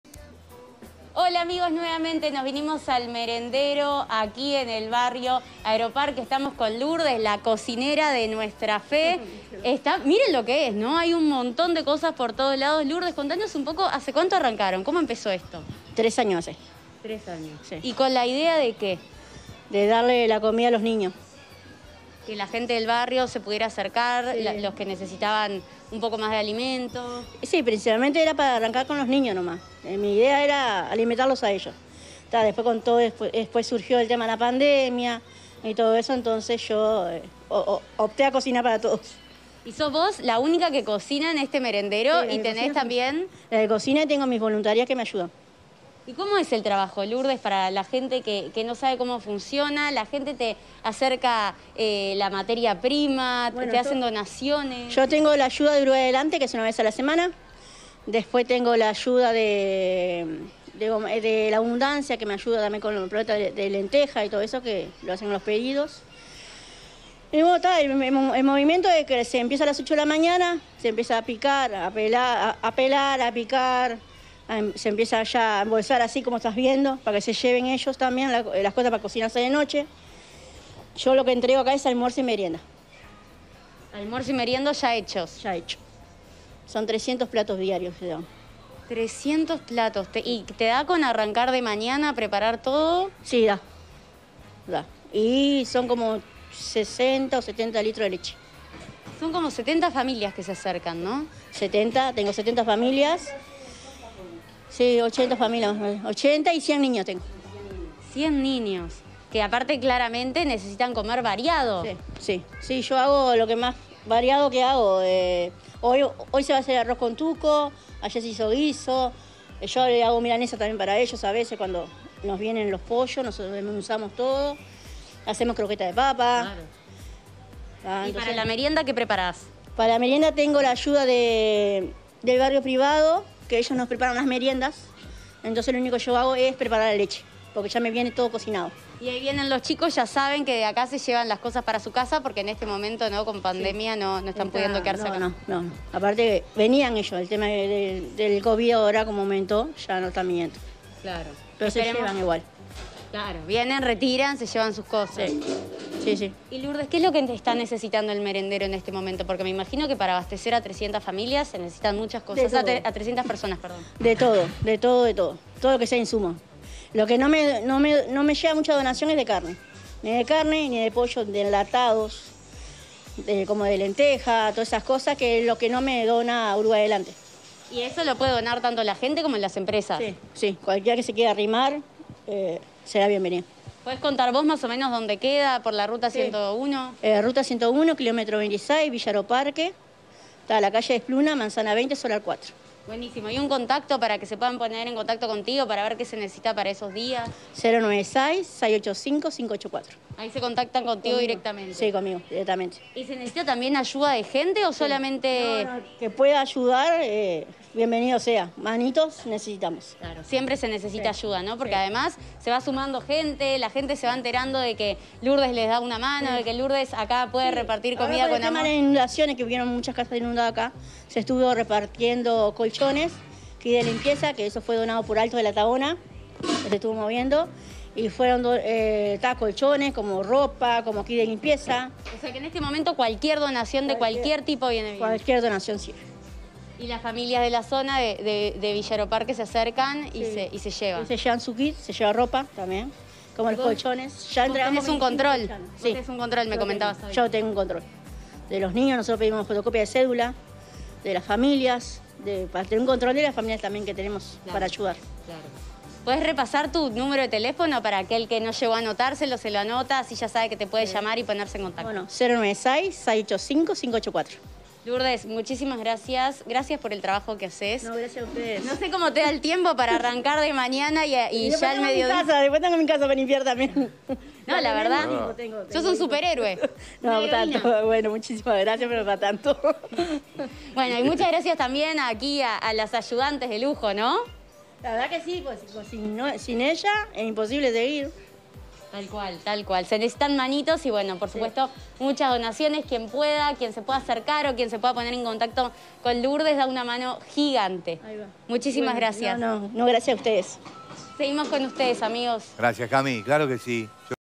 "Nuestra Fe": Merendero que alimenta a 70 familias y 100 niños
Este viernes Cami visitó la casa de Lourdes Muiños y su familia, que llevan el merendero "Nuestra Fe". Conocimos su historia desde que comenzó a repartir alimentos hace tres años hasta hoy, que le brinda almuerzo y merienda a niños del barrio Aeroparque de lunes a viernes.